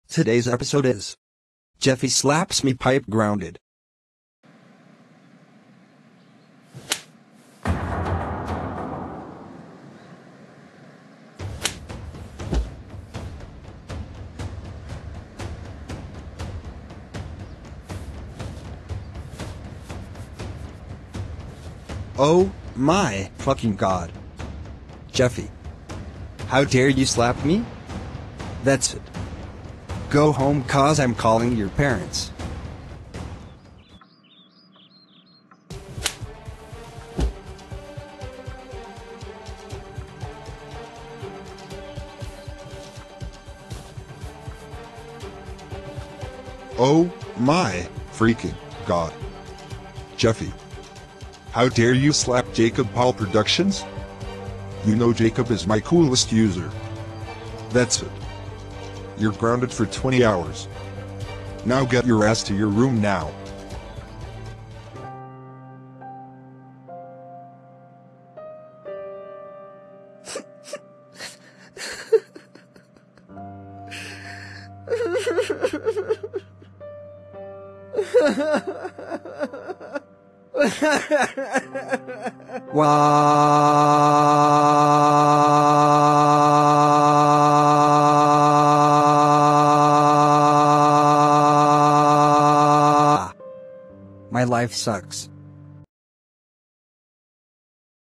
Jeffy Slaps Me & Gets Grounded
Note: I Like Jeffy but I made grounded videos out of him!!